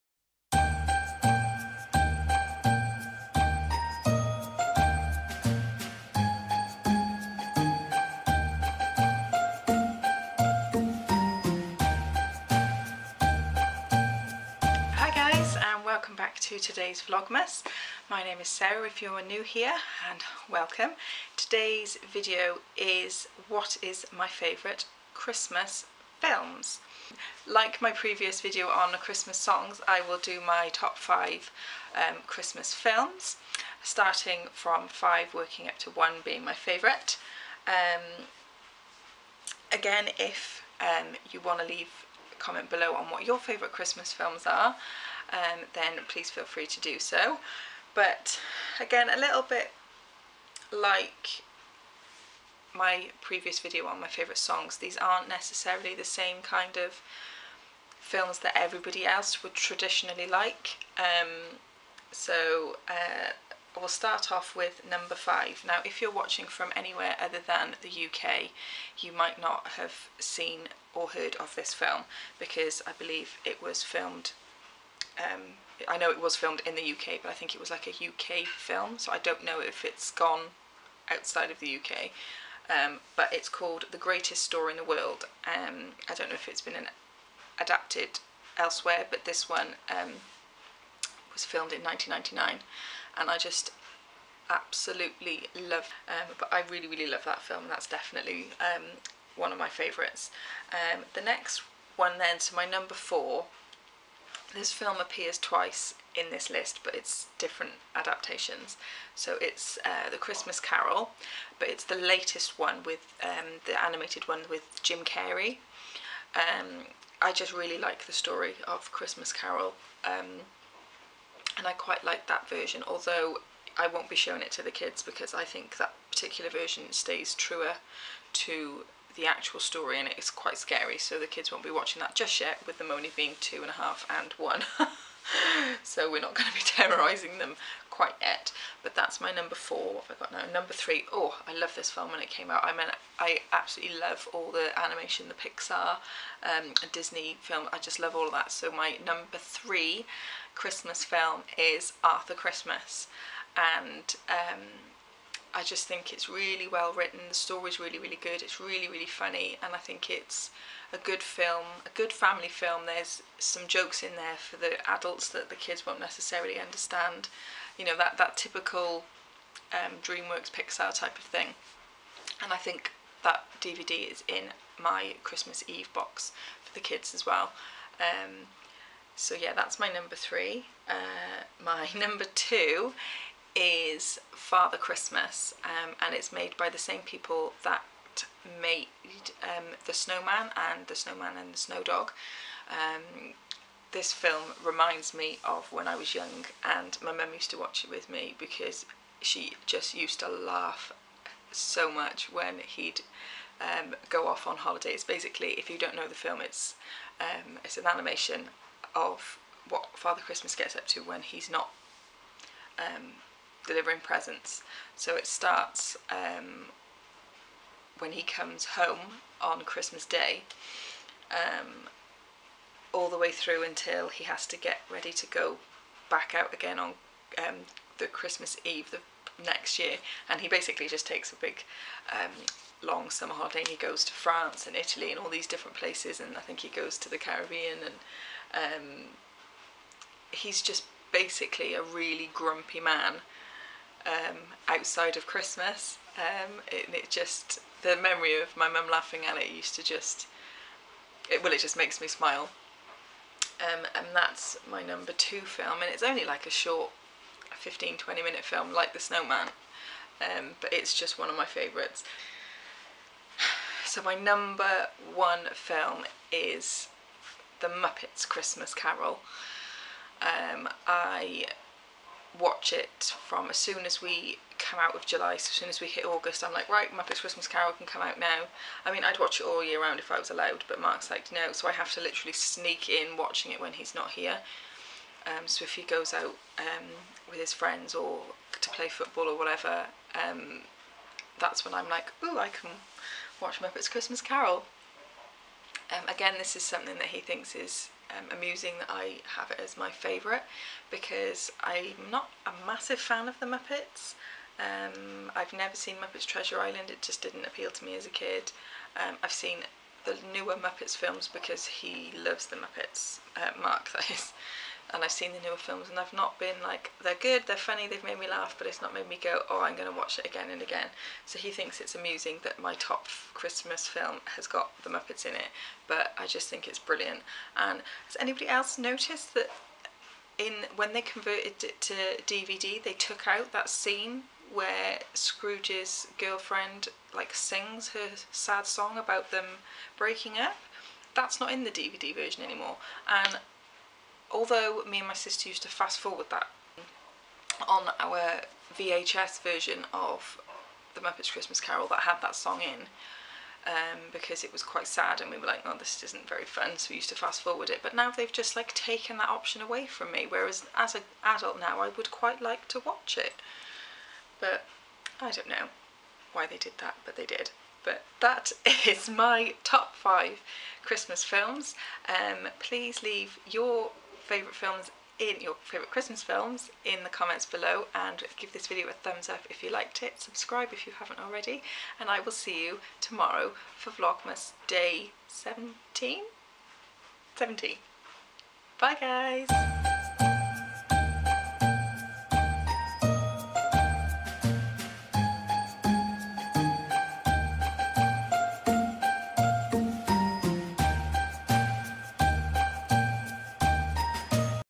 Vlogmas Day 16 | My Favourite Christmas Films | Mrs Dunn
I have listed my top 5 favourite Christmas films, let me know below what yours are!

VLOGMAS videos will be uploaded DAILY throughout vlogmas at 7:30pm (GMT)